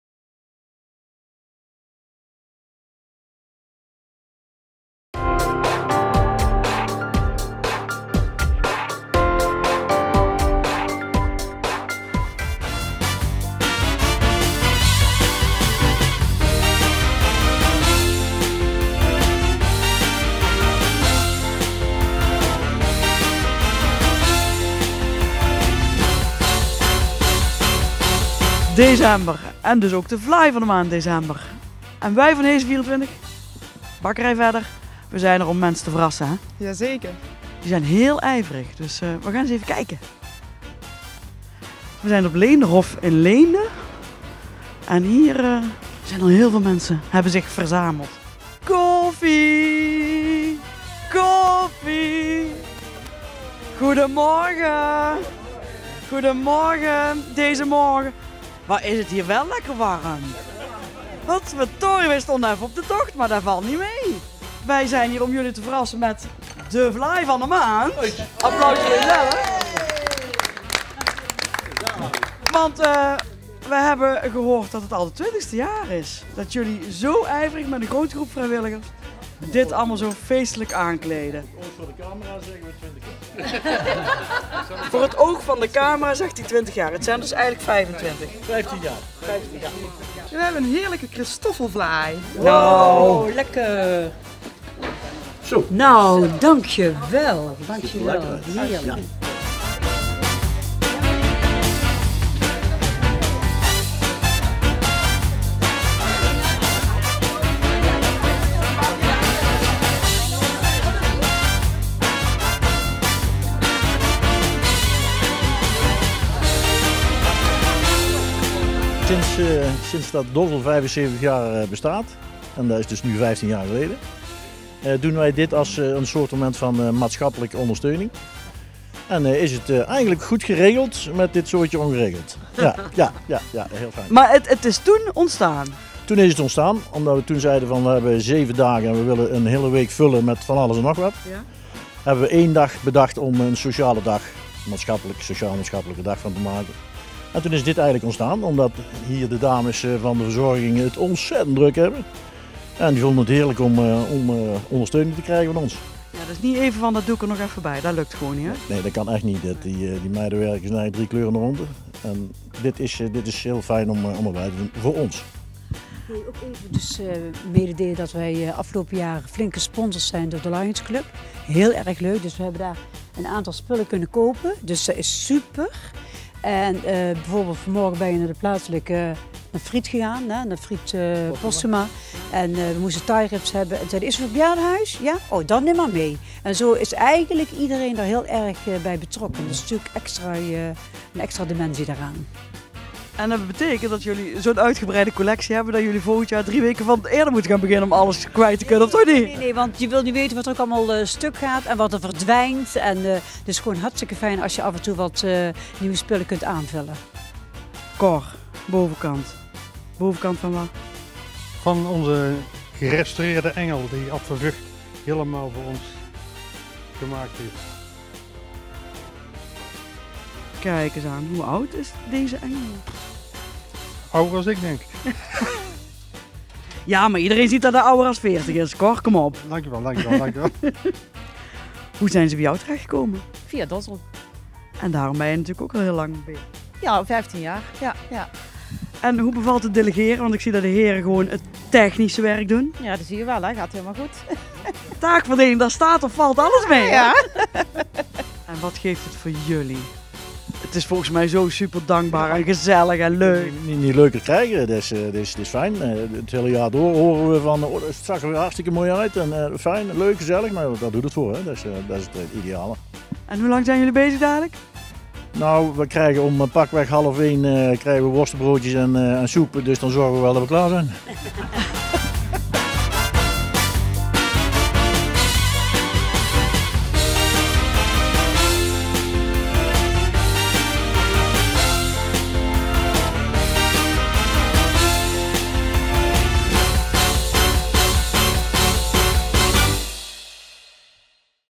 Vlaai van de maand december uitgereikt in Leende
LEENDE - Zaterdagmorgen werd de vlaai van de maand december uitgereikt in Leende. Daar waren vrijwilligers van DOSL voor de vijftiende keer bezig in Leenderhof om de gemeenschappelijk ruimtes in kerstsferen te brengen. Dit jaar een extraatje voor de vrijwilligers die van de Rotary maar liefst vijfhonderd euro ontvingen om extra inkopen te doen zoals kerstballen en kerstverlichting.